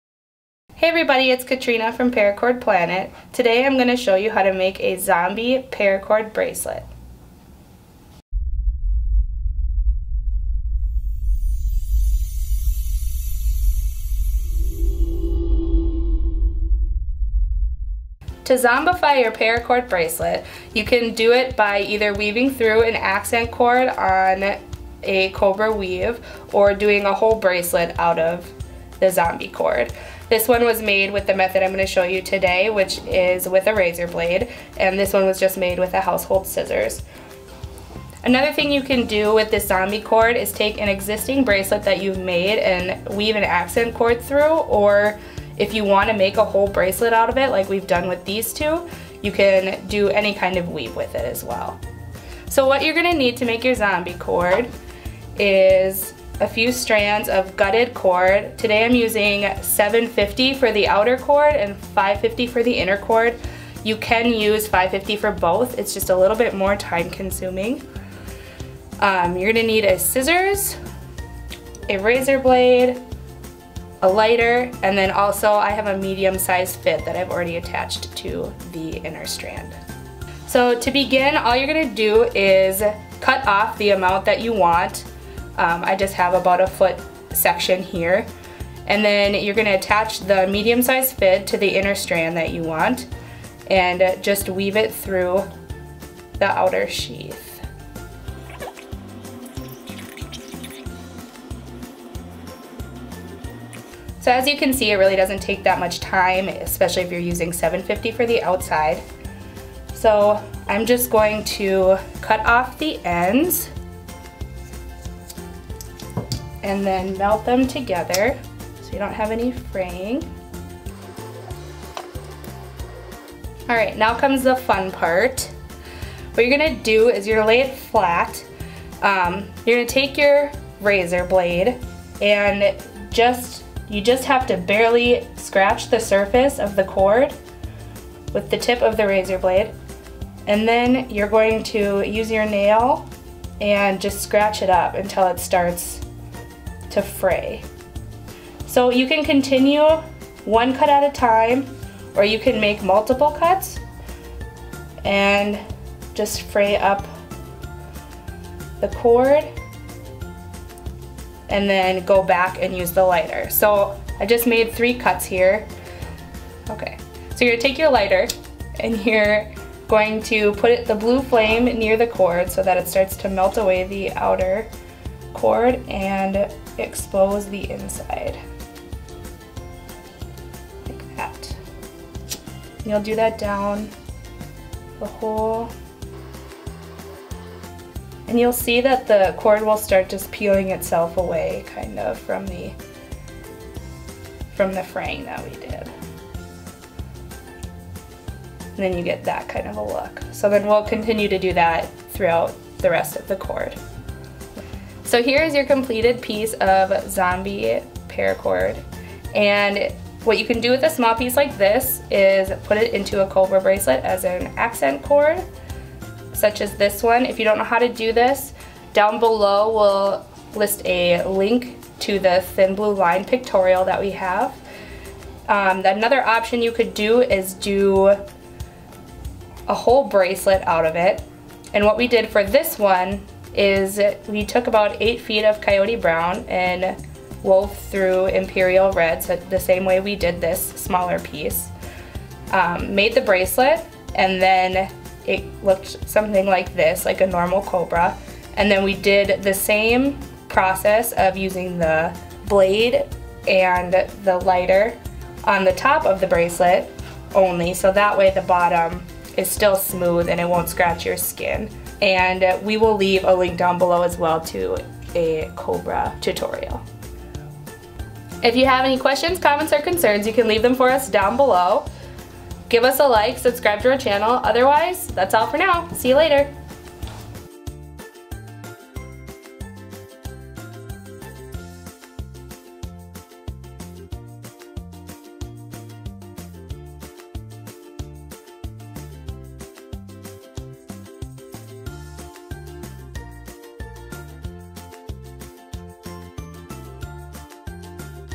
Zombify your Paracord Bracelet
Zombify your paracord bracelet! Learn how to make a Zombie paracord bracelet or make your current bracelet zombie theme! These unique bracelets are fun for Halloween, zombie pub crawls, and zombie TV show viewings.

They're easy to make, can be done with almost any weave, and can be done with household scissors or razor.